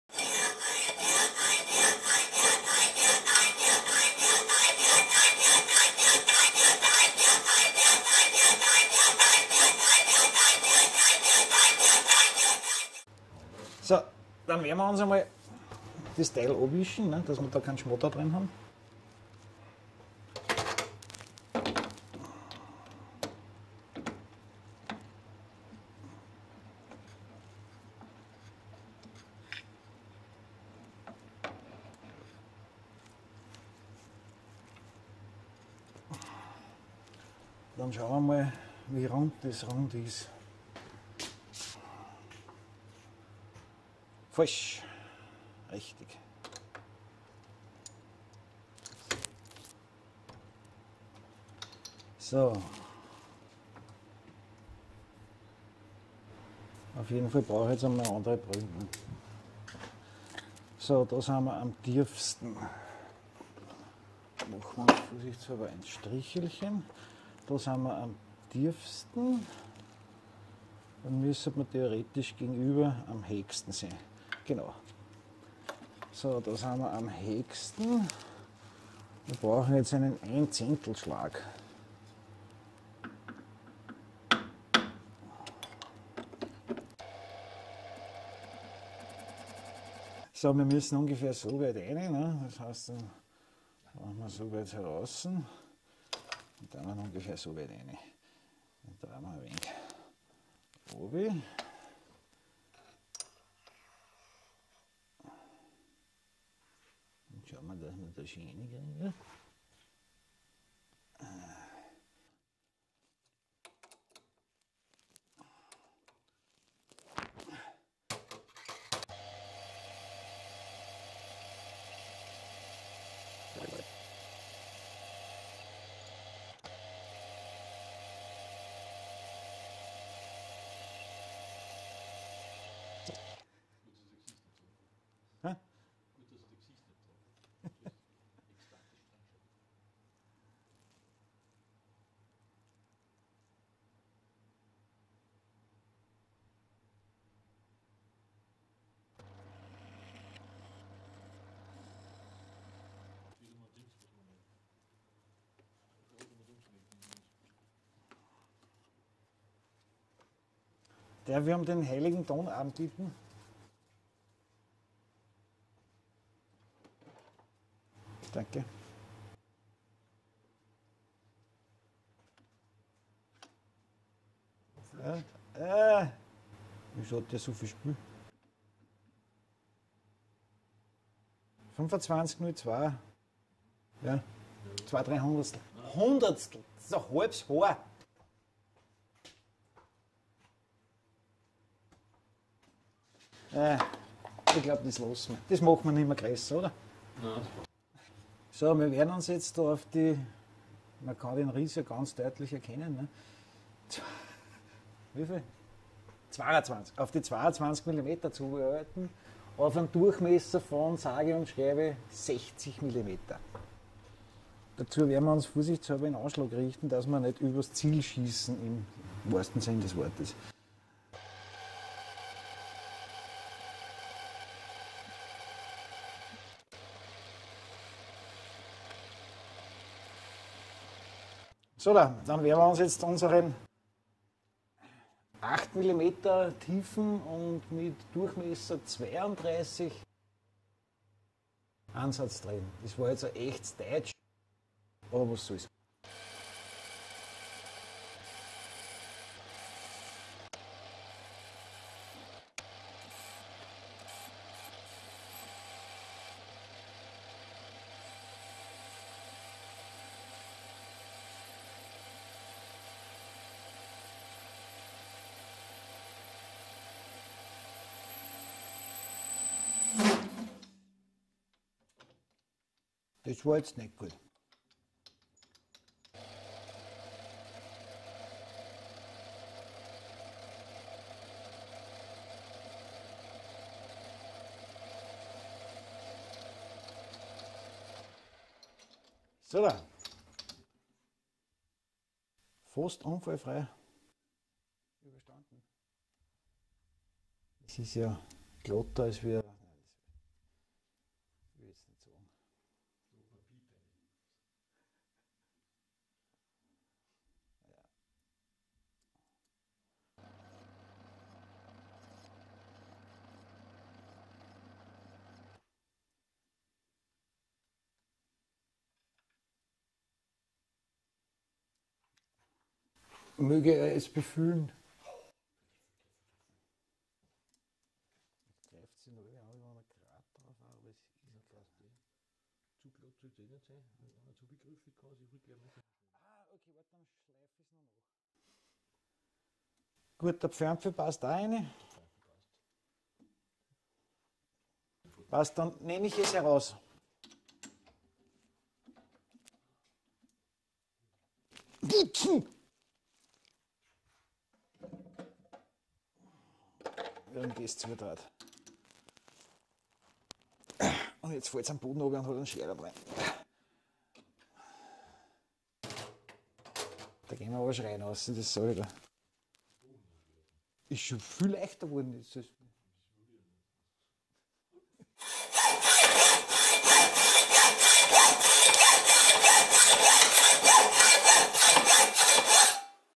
Tonarmträger aus Edelstahl für Plattenspieler, Teil 3, Oberseite bohren & drehen, Bernardo P 600 G
Zusätzlicher Tonarmträger aus Edelstahl für einen Plattenspieler aus 1.4305 Edelstahl. Teil 3; Oberseite Stufen drehen und Bohrung ausdrehen, auf einer Bernardo Profi 600 G.